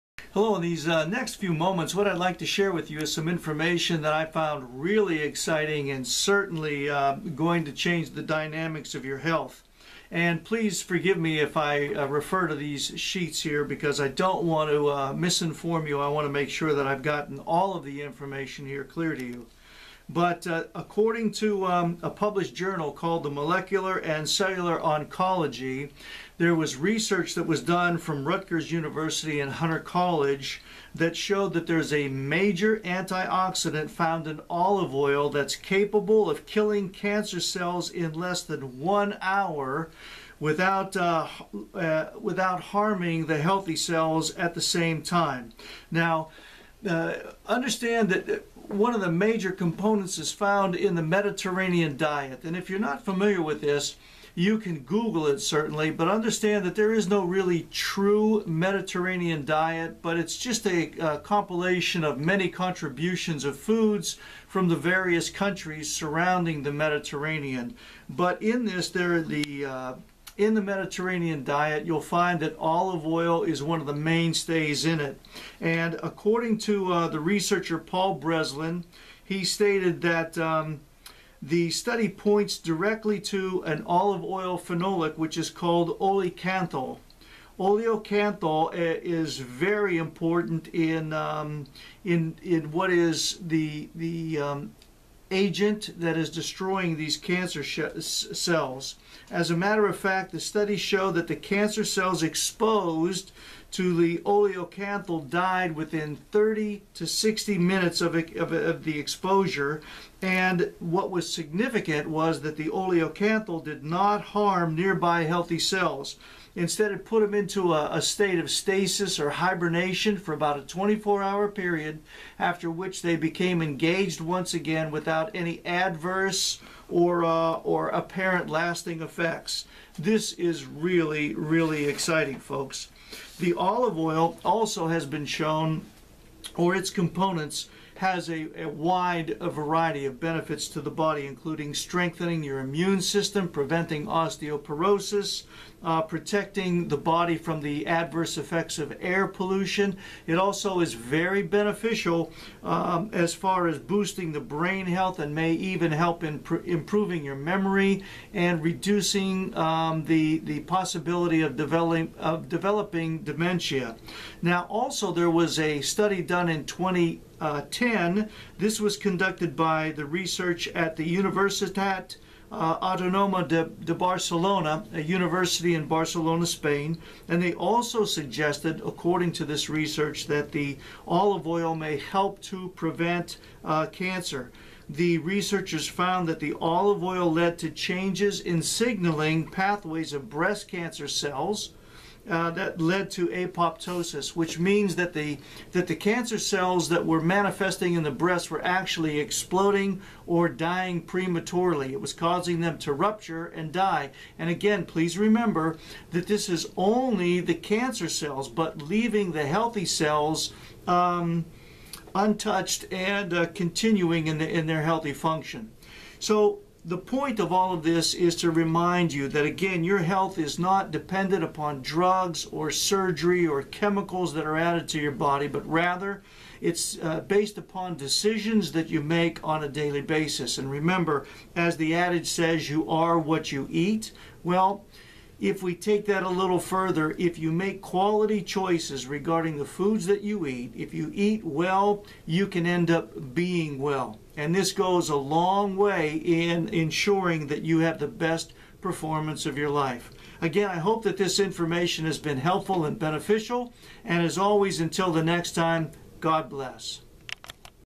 Kill Cancer Cells in 30 to 60 Minutes/Pro Active Wellness & Injury Centre
Exciting information is shared regarding the use of olive oil to literally destroy cancer cells!  We're not advocating in this presentation that olive oil cures cancer but, the research does indicate that the usage of olive oil in one's diet may destroy developing cancer cells.  This video shares this remarkable news of hope in the fight against cancer!

Pro-Active Wellness & Injury Centre
1207 E Main
Danville, IL 61832